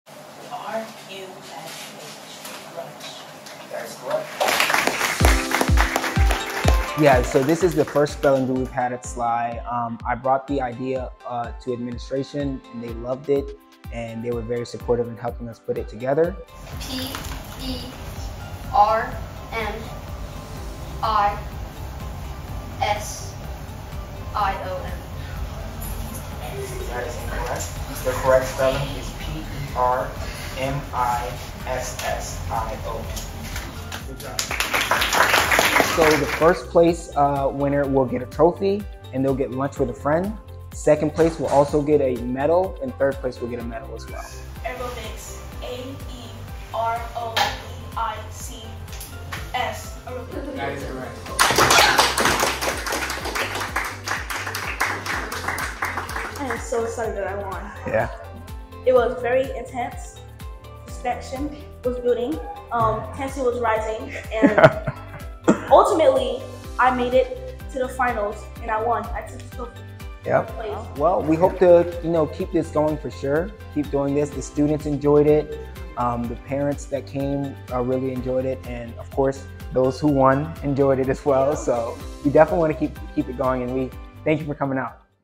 Battle of the Brains: Sligh Middle School Spelling Bee
39 Sligh Middle School students battled it out on Thursday during the school's first ever spelling bee 🐝
C-O-N-G-R-A-T-U-L-A-T-I-O-N-S to winner Josiah!